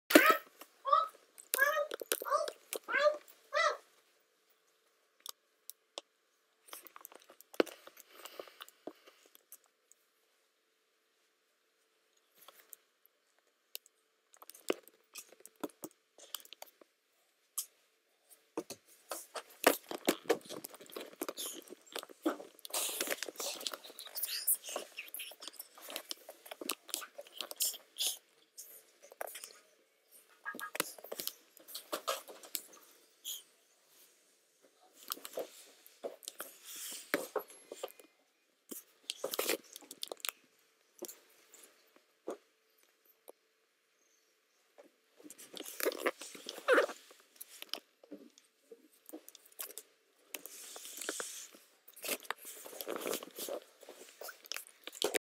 Blindman!
steph's little brother Jacob playing blindman!